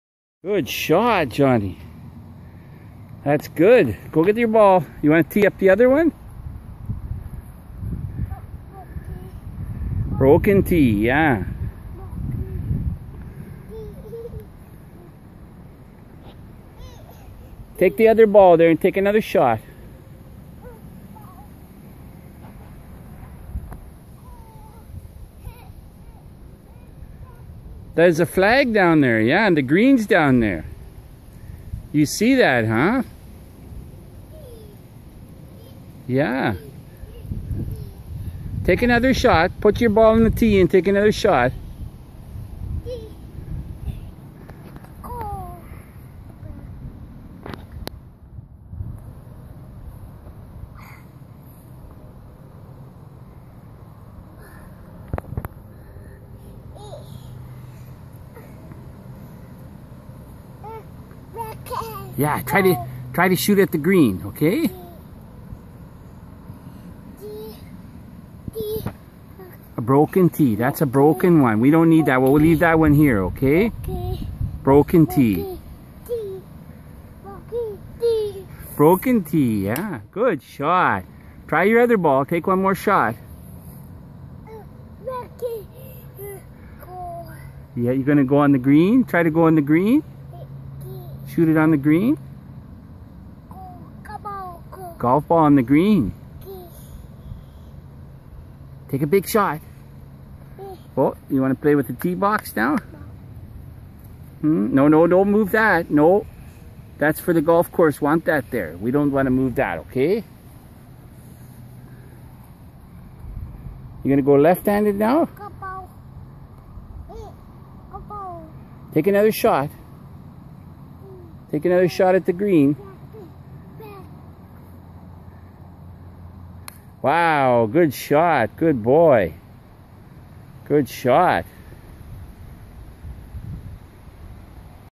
Jonathan golfing with dad at the falls golf club in chilliwack bc august 2014.
Golfing at the falls golf club in Chilliwack bc Canada.